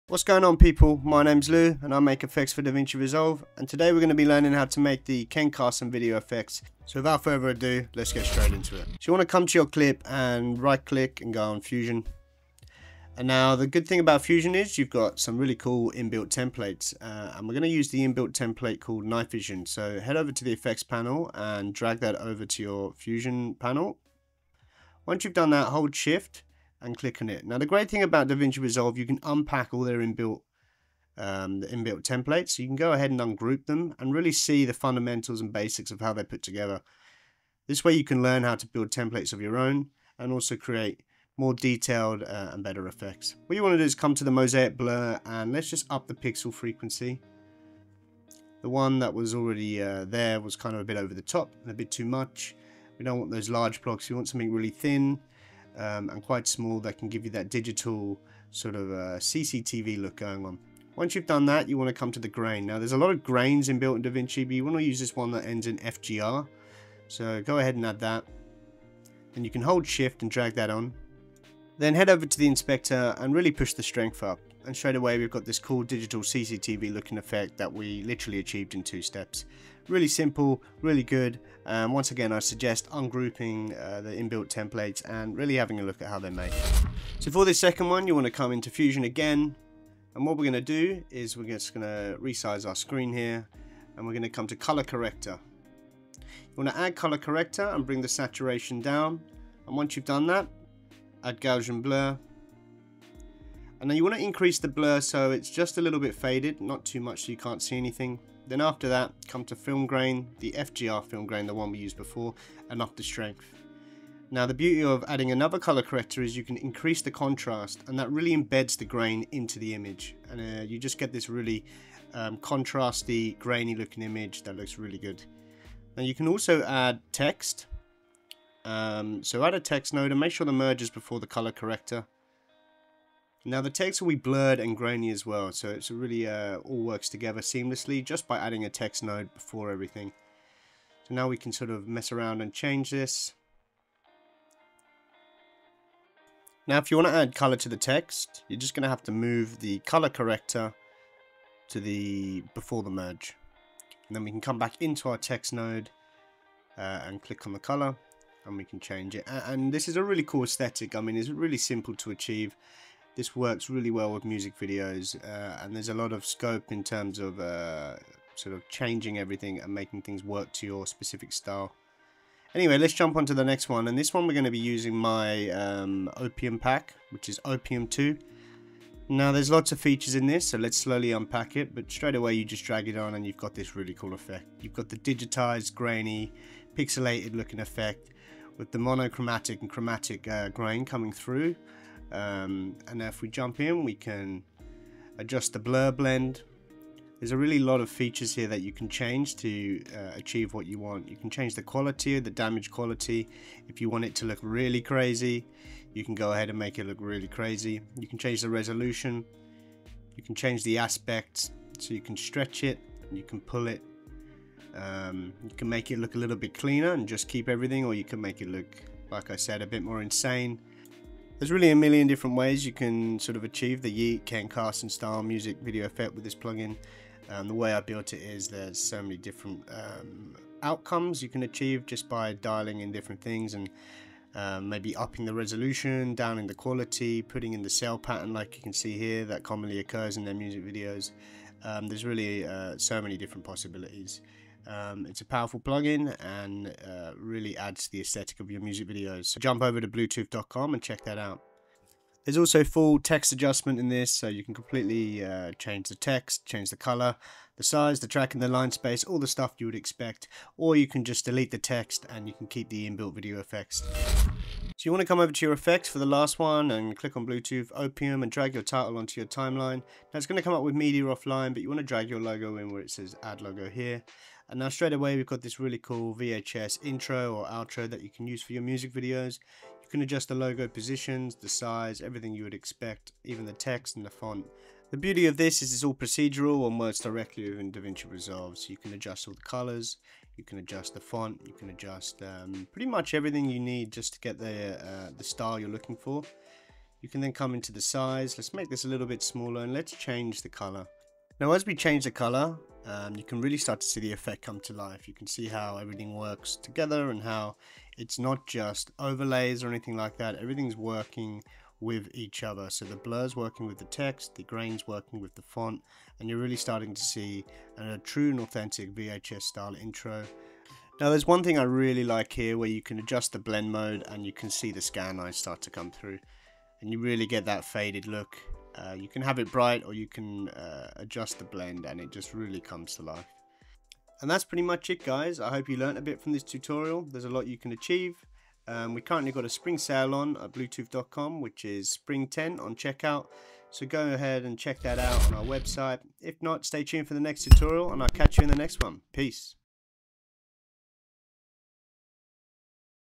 How to edit like KEN CARSON in Davinci Resolve
🎁 Download the KEN CARSON effect

Learn how to create Ken Carson-style VHS + glitch edits in DaVinci Resolve Studio—no plugins, no fluff. This step-by-step tutorial dives into wild analog textures, digital distortion, and raw visual energy. Perfect for rage edits, underground visuals, and hyperpop aesthetics, you’ll master the look of blown-out camcorder footage fused with chaotic, rhythmic glitch effects that snap on beat and scream underground culture.

🔧 What You’ll Learn:
Setting up Fusion for VHS + glitch workflows

🔗 Tools & Assets Used:
Fusion page in DaVinci Resolve Studio

Weekly DaVinci Resolve tutorials for glitch edits, rage visuals, VFX workflows, and analog-style grading.